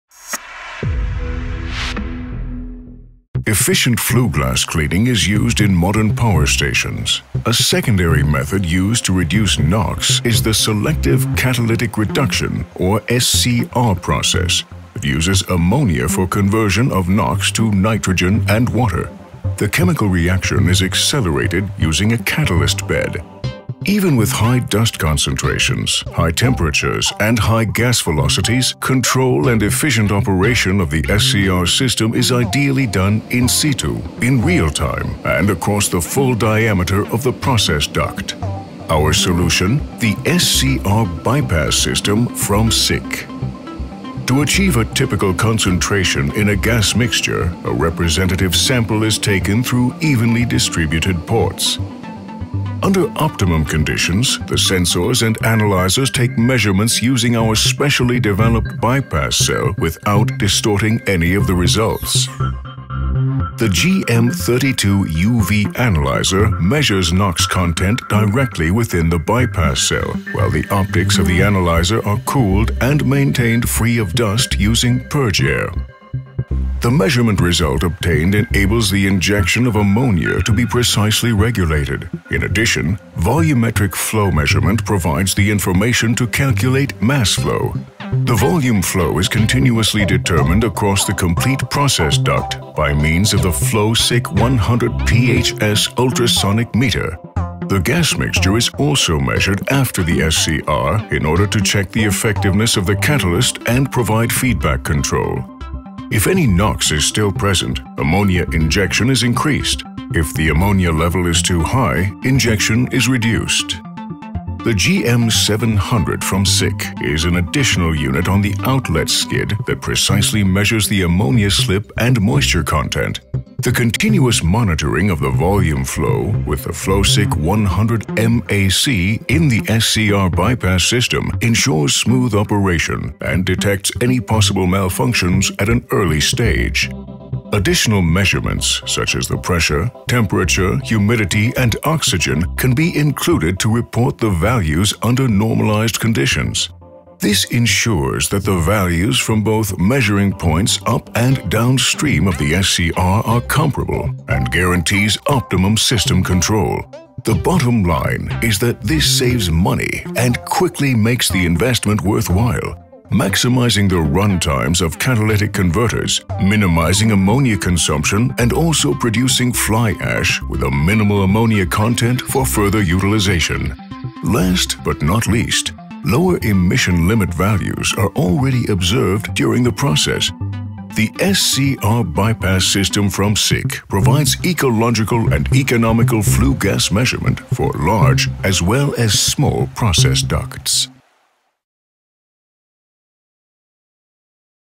NOx reduction with SCR (selective catalytic reduction) bypass system from SICK | SICK AG
Efficient flue gas cleaning is used in modern power stations. A secondary method used to reduce NOx is the SCR (selective catalytic reduction) that uses NH3 for conversion of NOx to nitrogen and water. Even with high dust concentrations, high temperatures, and high gas velocities, control and efficient operation of the SCR system is ideally done in-situ, in real-time, and across the full diameter of the process duct. Our solution: the SCR Bypass System from SICK.